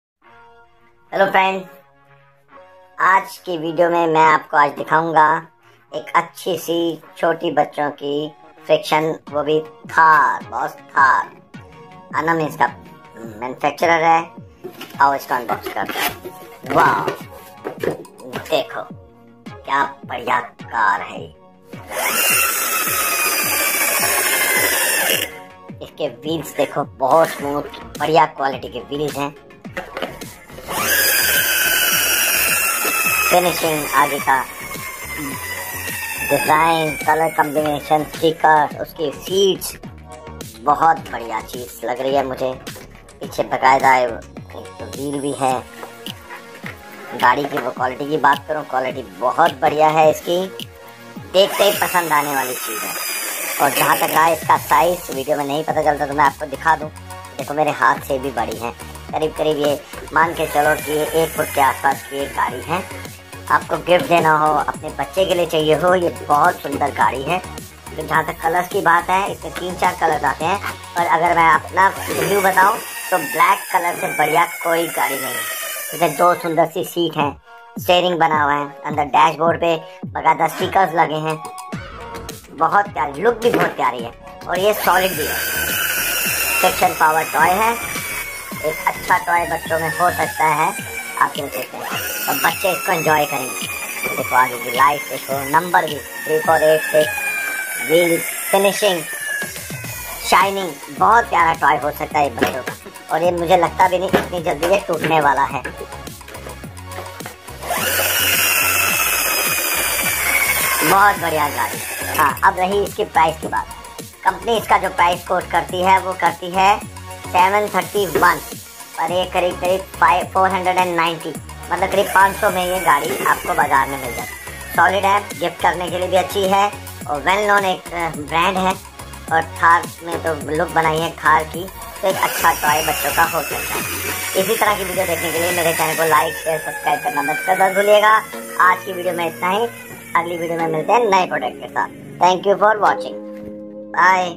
Thar Toy car. Thar Toy Car.
Thar Toy Car. A friction toy car for kids. A friction toy available in various clolours like white, black, etc.

A solid first class plastic ki bani hui ek acchi toy friction toy car hai.

Most trending toy car in 2024 available at a just Rs. approx 500/-.

Plz follow our channel for the latest update on toys for your loveones at a very reasonable prices. 🙏🙂